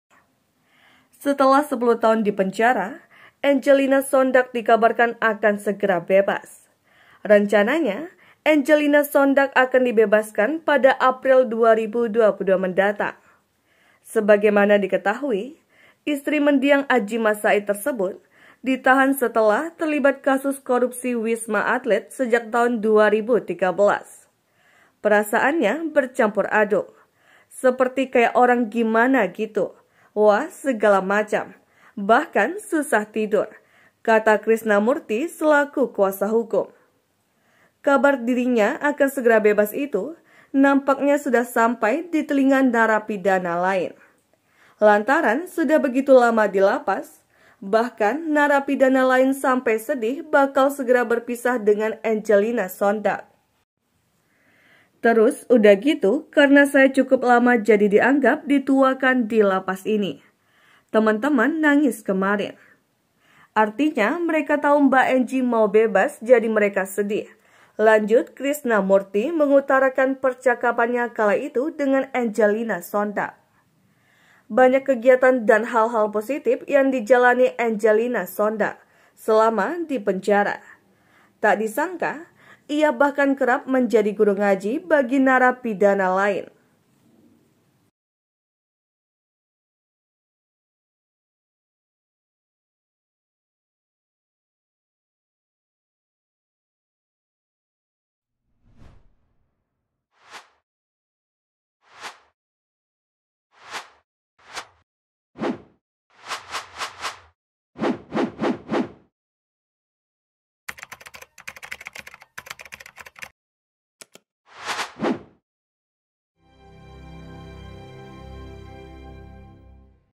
🔴Angelina Sondakh Akan Bebas Malah Ditangisi Napi Lain, Kebaikannya selama 10 Tahun di Lapas Terkuak
Setelah 10 tahun di penjara, Angelina Sondakh dikabarkan akan segera bebas.

Rencananya, Angelina Sondakh akan dibebaskan pada April 2022 mendatang.

Sebagaimana diketahui, istri mendiang Adjie Massaid tersebut ditahan setelah terlibat kasus korupsi Wisma Atlet sejak tahun 2013.

"Perasaannya bercampur aduk, seperti kayak orang gimana gitu, wah segala macem. Bahkan susah tidur," kata Krisna Mukti selaku kuasa hukum Angelina Sondakh